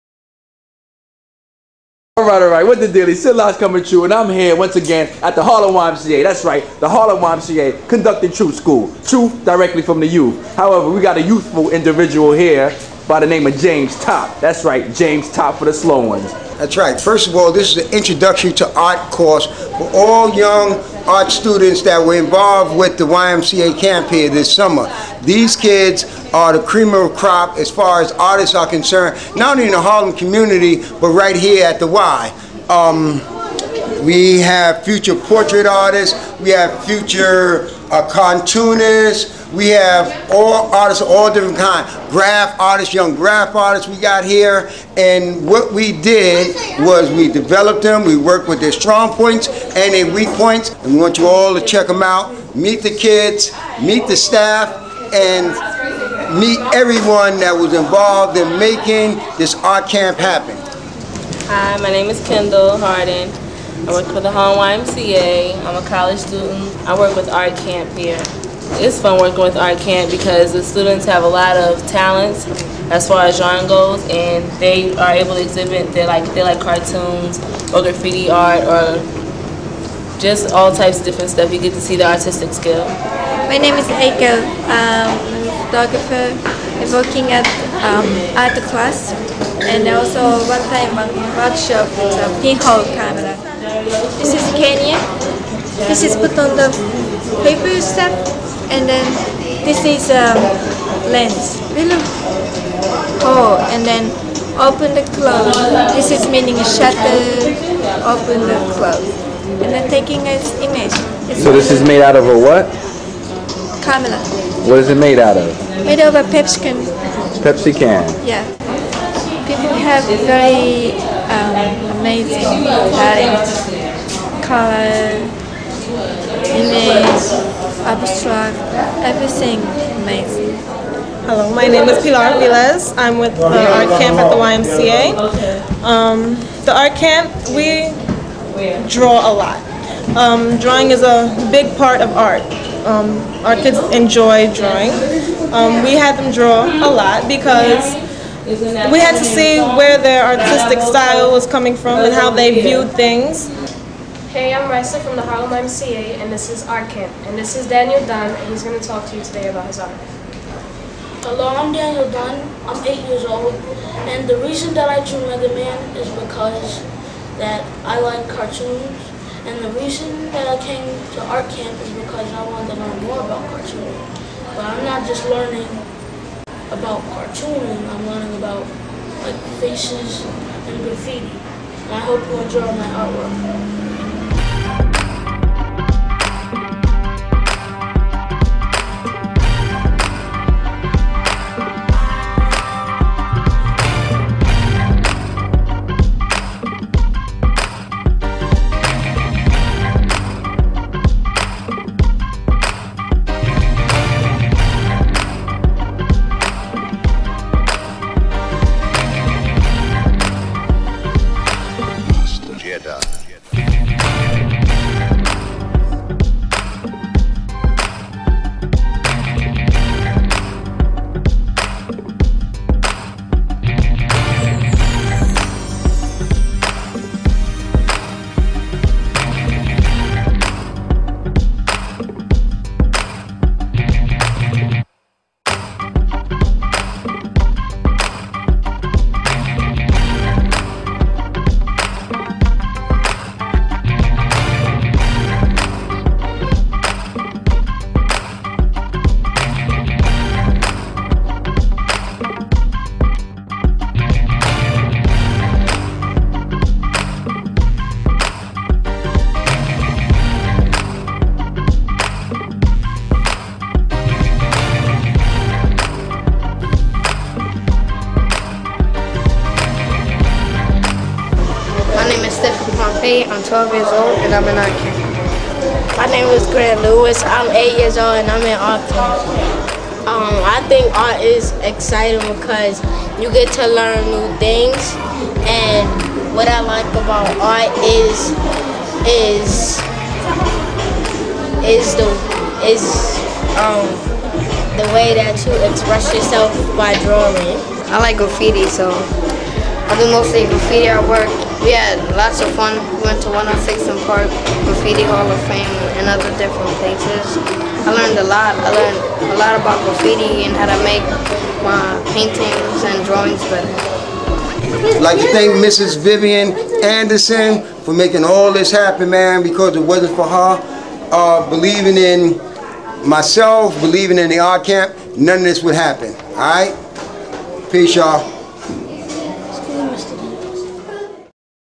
Tru Skool TV "Tru Art" with James Top by SidLocks
James Top gives you the oppurtunity to share his experience teaching the youth how to draw and appreciate art.